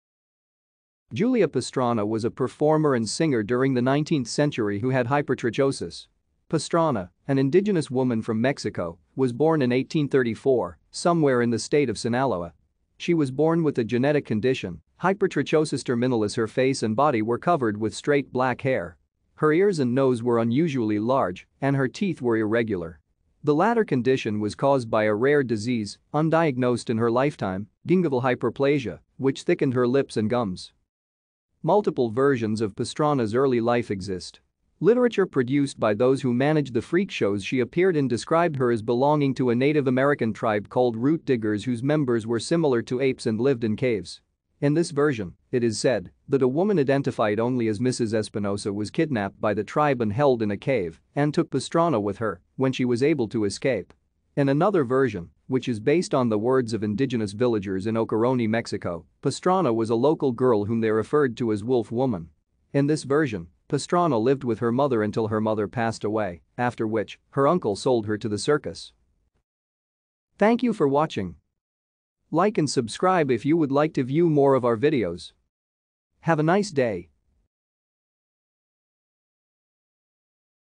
Julia Pastrana
Julia Pastrana  was a performer and singer during the 19th century who had hypertrichosis. Pastrana, an indigenous woman from Mexico, was born in 1834, somewhere in the state of Sinaloa. She was born with a genetic condition, hypertrichosis terminalis ; her face and body were covered with straight black hair. Her ears and nose were unusually large, and her teeth were irregular. The latter condition was caused by a rare disease, undiagnosed in her lifetime, gingival hyperplasia, which thickened her lips and gums.